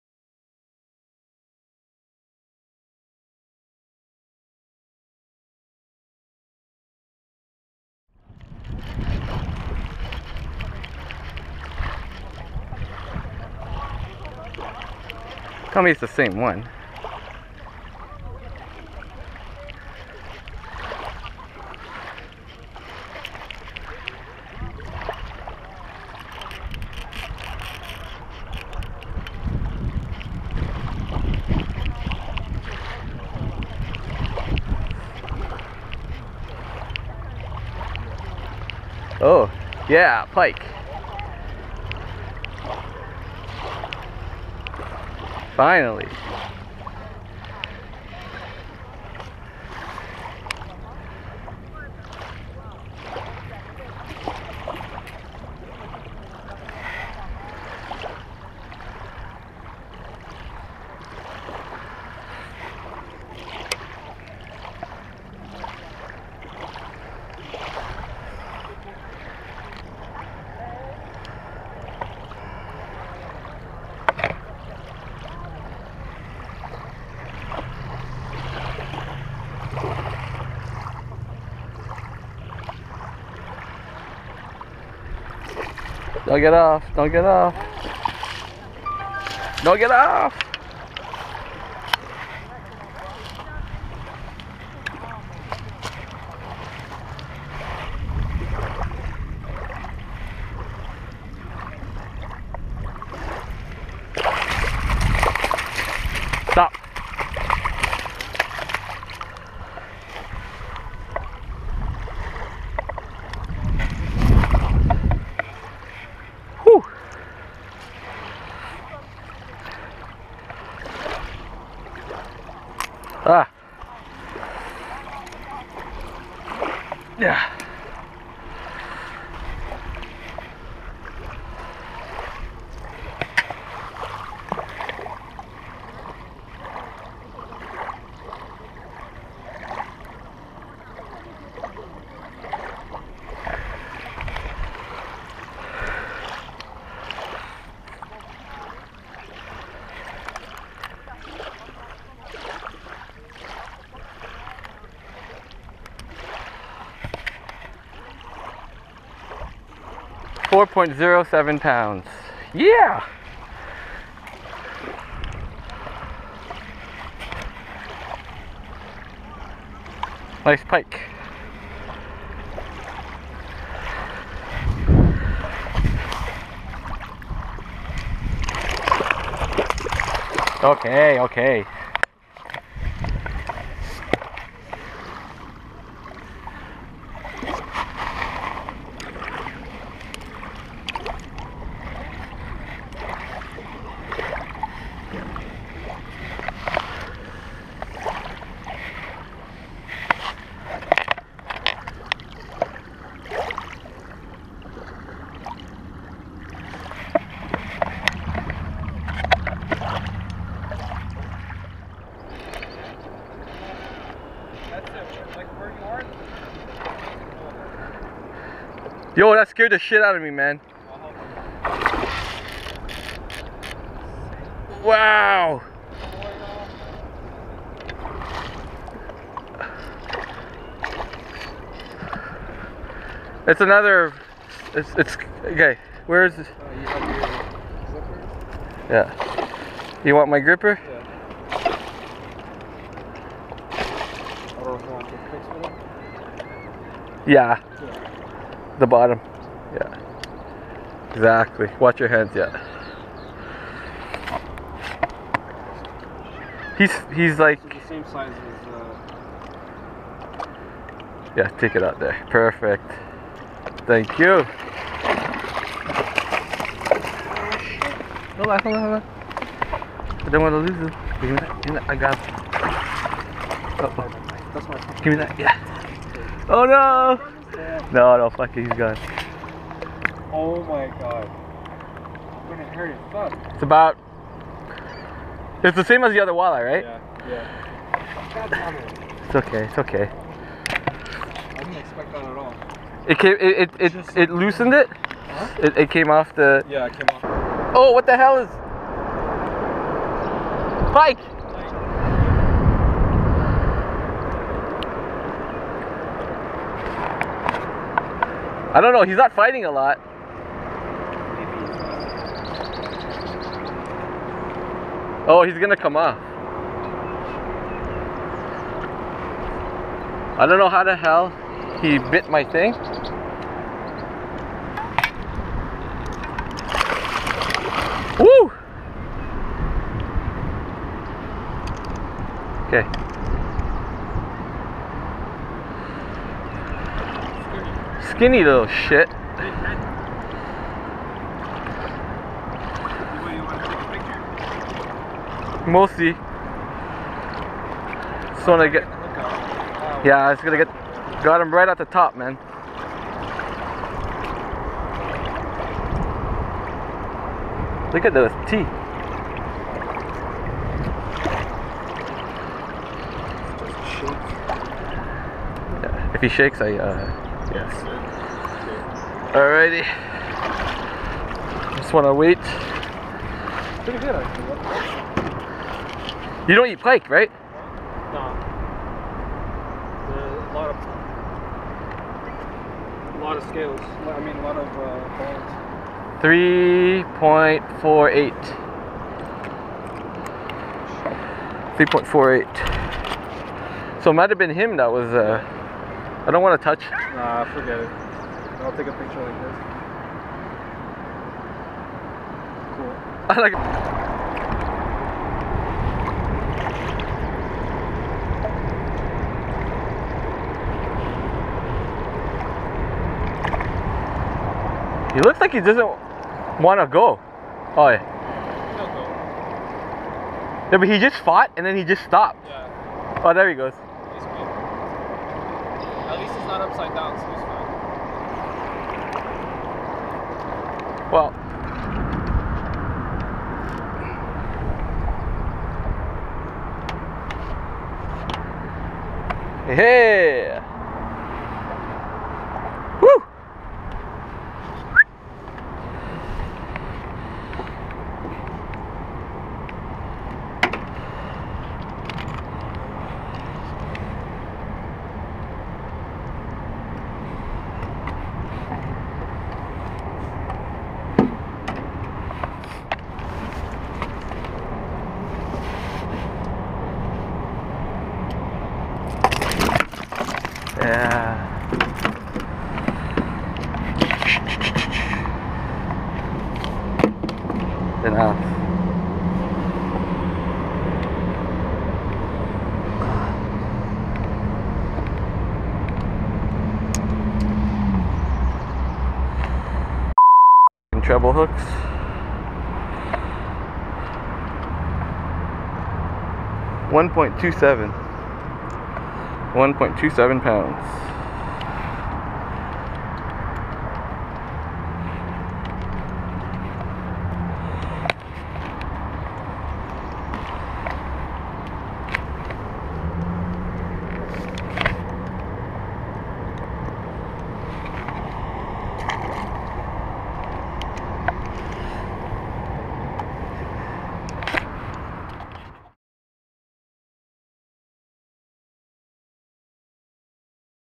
Pike Fishing May 2022
Catching some nice Pike!

Equipment:
Shimano Caenan reel on 6'7" MH Rapala Aeris rod on 15 lbs Berkley Big Game mono.
Daiwa Strikeforce reel on 6'7" MH Rapala Aeris rod on 15 lbs Berkley Big Game mono.

ReelSonar Digital Fish Scale